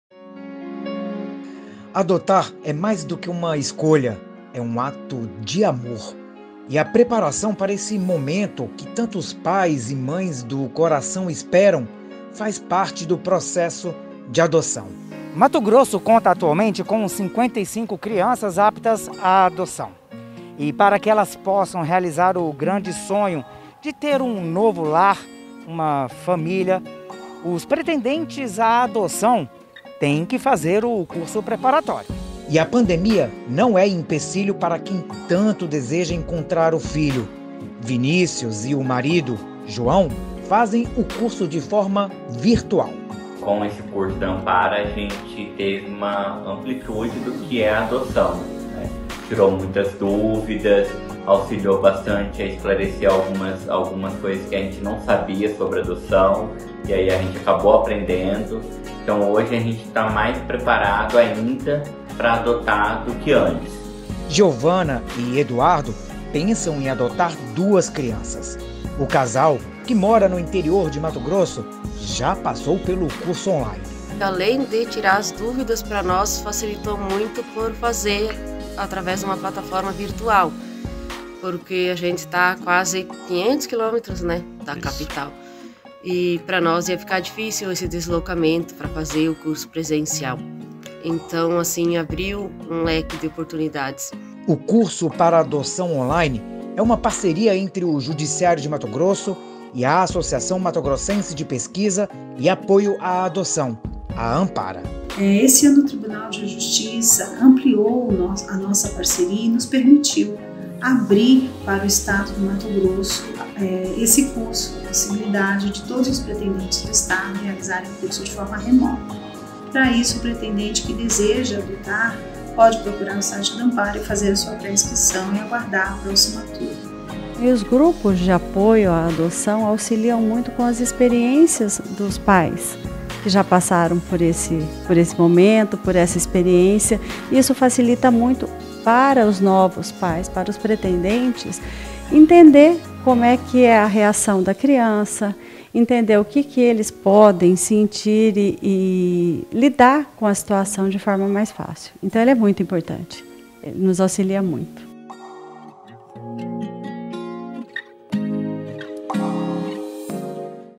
Judiciário oferece curso virtual e gratuito para pretendentes à adoção de todo MT
Quem deseja adotar precisa passar por um curso de preparação.  Para não atrasar o processo de adoção, o Judiciário Mato-grossense em parceria com a Associação Matogrossense de Pesquisa e Apoio à Adoção (AMPARA) está realizando o curso preparatório de Adoção de forma virtual.

As aulas estão disponibilizadas gratuitamente na plataforma da Ampara para todos os municípios de Mato Grosso.

Confira ▶️